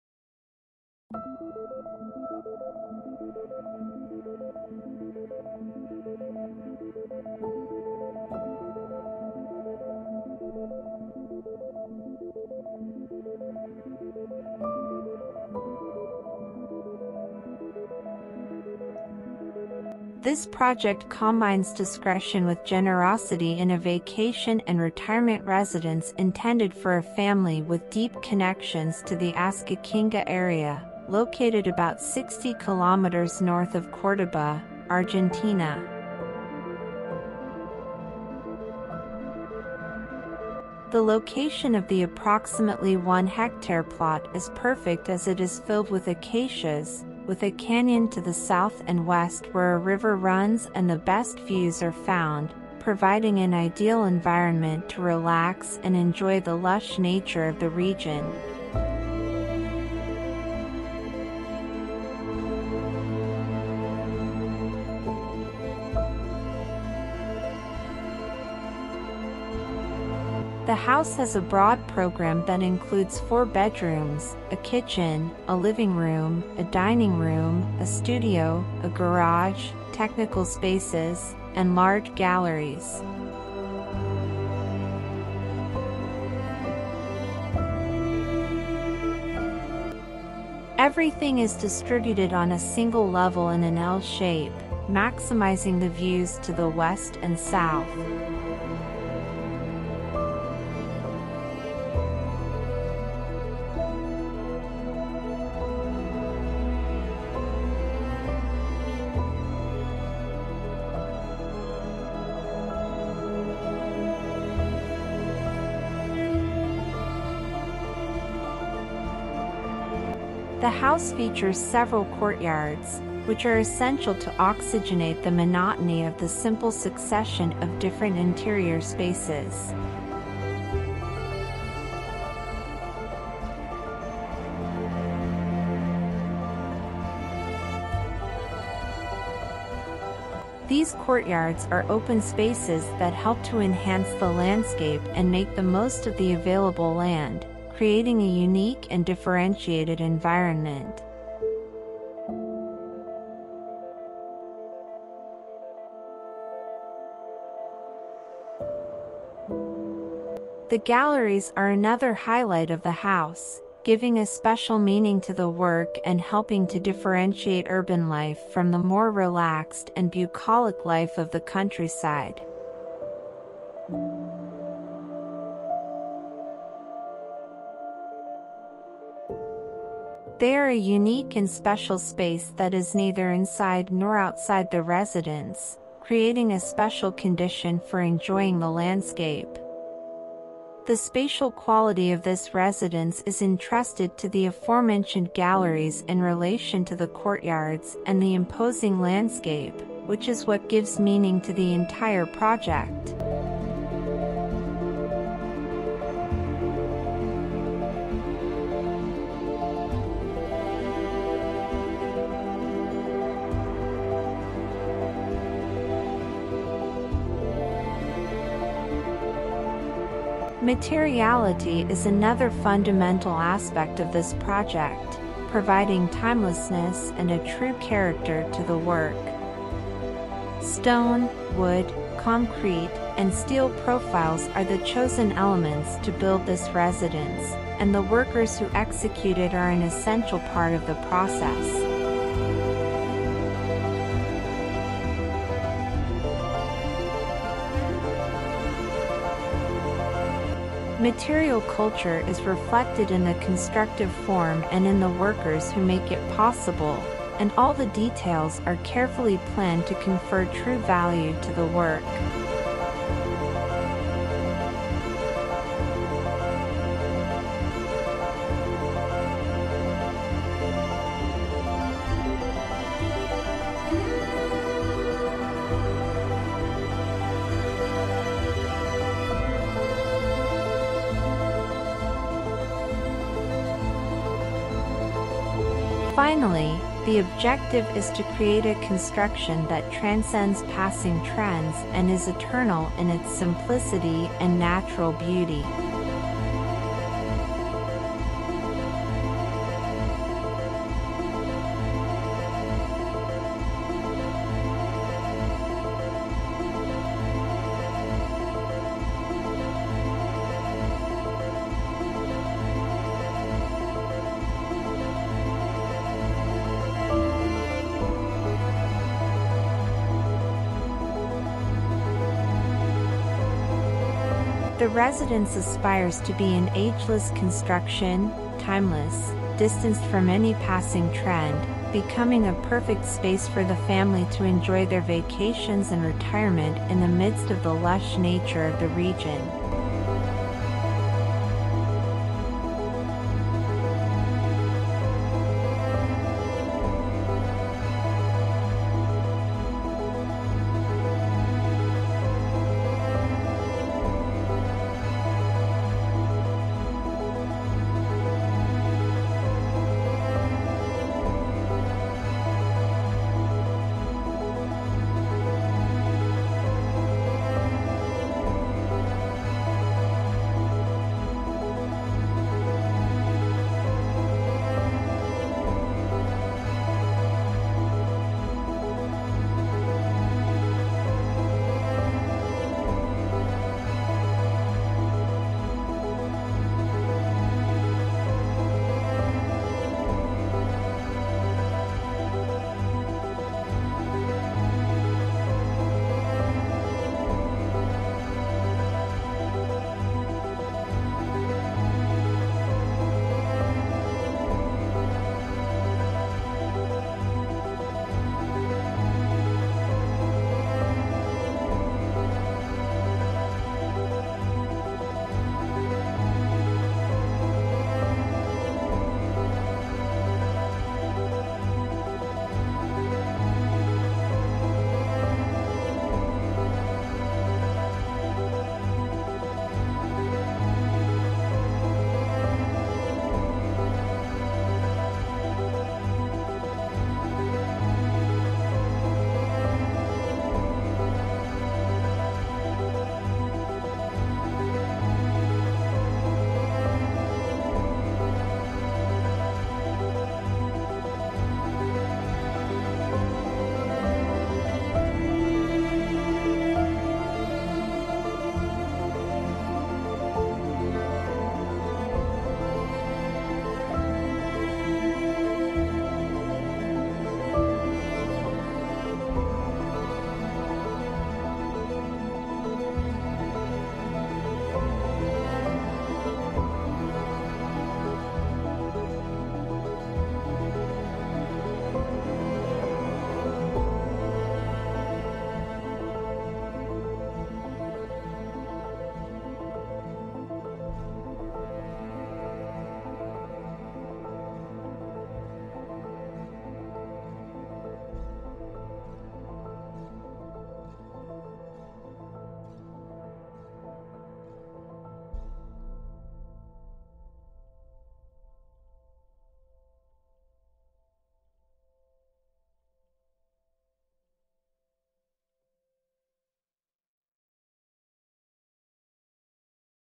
Stunning Vacation and Retirement Home in Argentina's Countryside
This video showcases a stunning vacation and retirement home located amidst the lush countryside of Ascochinga, Argentina. With an area of approximately 1 hectare, the house offers breathtaking views of acacias, a gorge, and a river. The project was designed with the aim of combining discretion and generosity, providing residents with a peaceful and welcoming environment. Featuring four bedrooms, a kitchen, living room, dining room, studio, garage, technical spaces, and large galleries, all distributed on a single "L" shaped level, the house was built with timeless materials and carefully planned to add real value to the work. This is a perfect retreat for those seeking relaxation and contact with nature in a unique and differentiated environment.

Location: Ascochinga, Argentina
Architects: MWS arquitectura; MWS arquitectura
Area: 750 sqm
Year: 2016
Photography: Gonzalo Viramonte